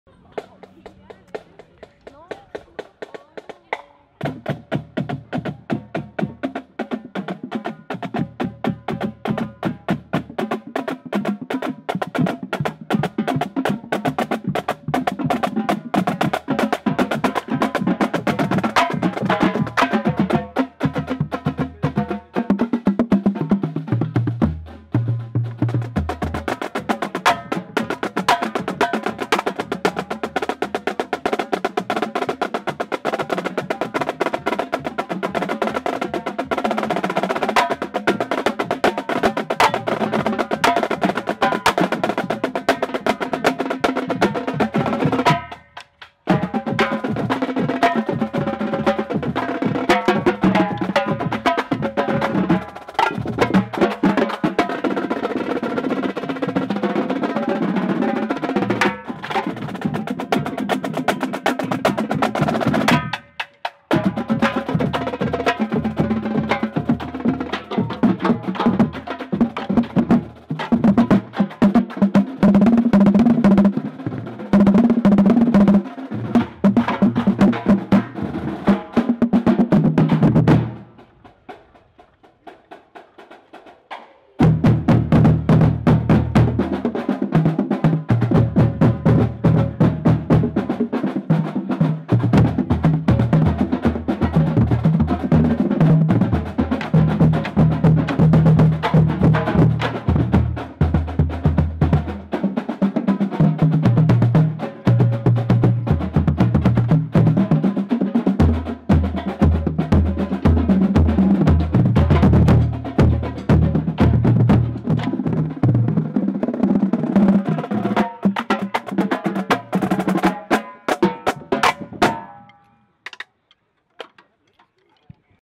Drumline Cadence - Goshen Crimson Marching Band 2019
The drumline from the Goshen Crimson Marching Band march to Foreman Field(Goshen High School football field) prior to the August 30th, 2019 home football game.  This is a tradition where they march in formation and play cadence the whole way.